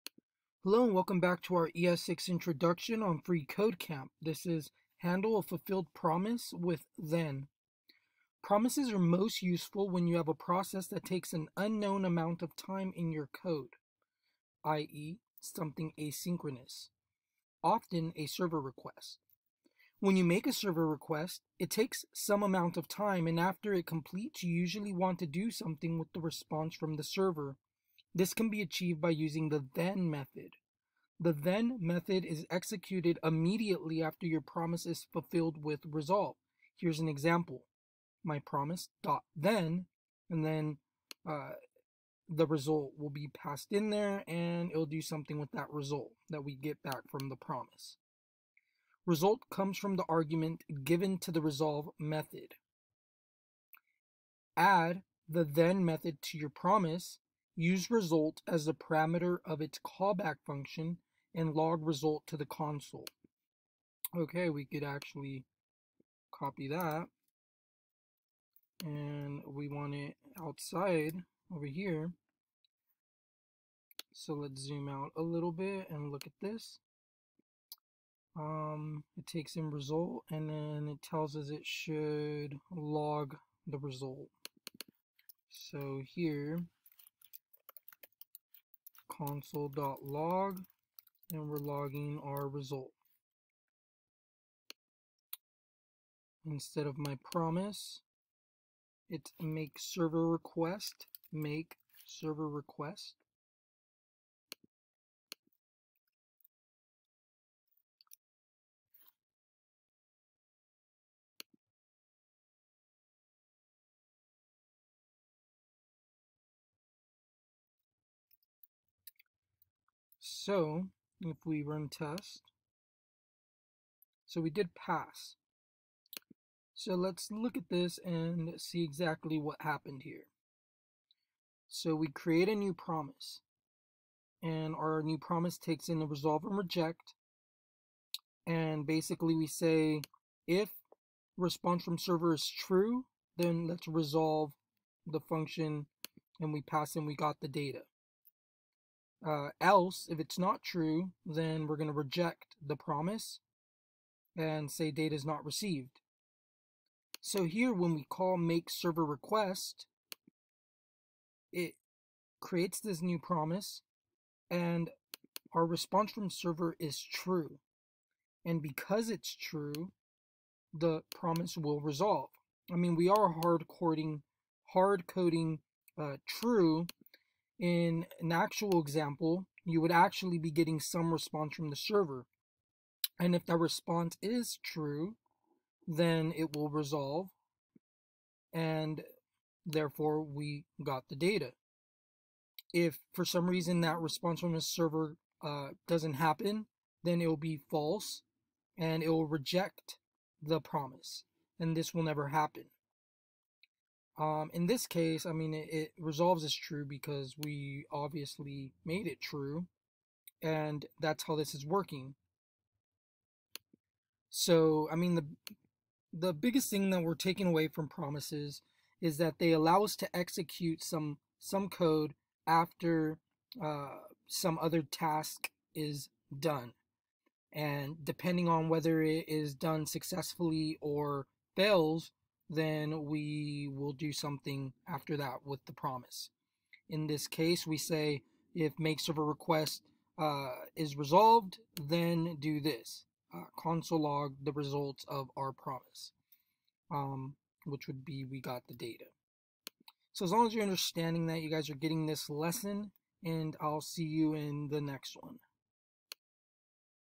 Handle a Fulfilled Promise with then, freeCodeCamp's ES6 Challenges
In this lesson from freeCodeCamp's ES6 Challenges, we look at handling a fulfilled promise in javascript.  In the previous lessons, we have been learning about promises and how they wait for some code to finish running then we can do something after that promise is either resolved or rejected.  In this lesson, we learn about the .then syntax which allows us to do something once our promise is resolved.  Learning promises is key for understanding how to handle requests and other instances involving asynchronous programming.